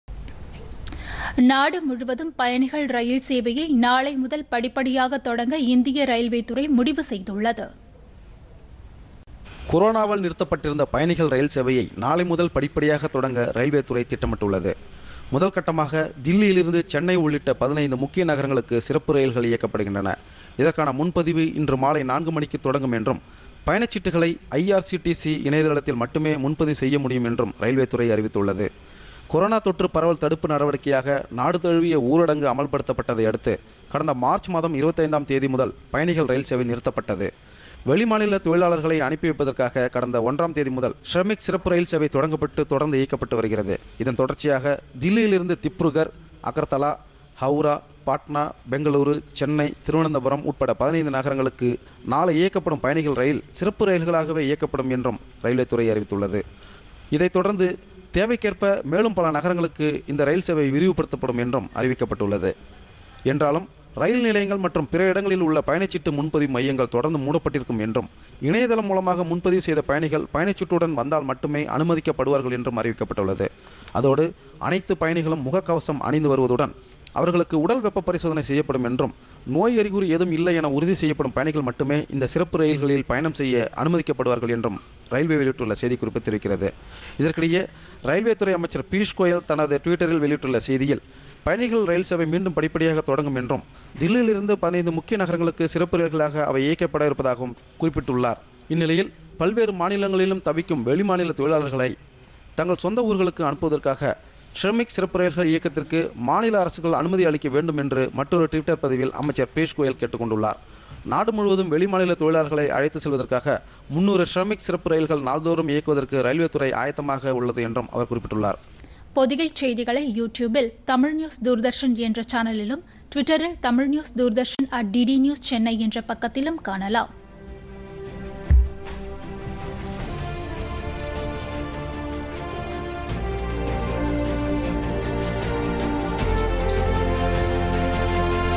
படிப்படியாக பயணிகள் ரயில் சேவை
தில்லியிலிருந்து, சென்னை உள்ளிட்ட 15 நகரங்களுக்கு நாளை பயணிகள் சிறப்பு ரயில்களை இயக்குகிறது ரயில்வேத்துறை.
  ** படிப்படியாக ரயில் சேவை விரிவுபடுத்தப்படும் என அறிவிப்பு**Visit : TamilNews - Doordarshan